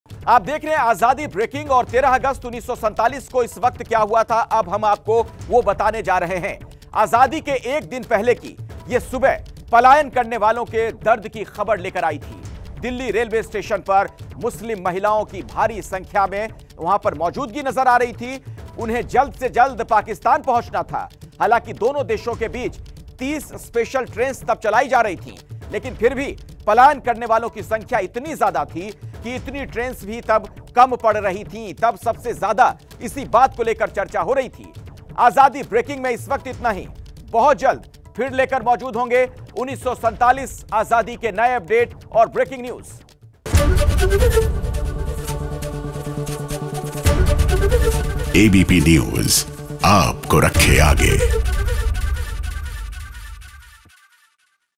13 Aug 1947, 5 AM : Delhi रेलवे स्टेशन पर उमड़ी मुसलमानों की भीड़ | आजादी ब्रेकिंग
News:
13 Aug 1947, 5 AM : Delhi रेलवे स्टेशन पर उमड़ी मुसलमानों की भीड़ | आजादी ब्रेकिंग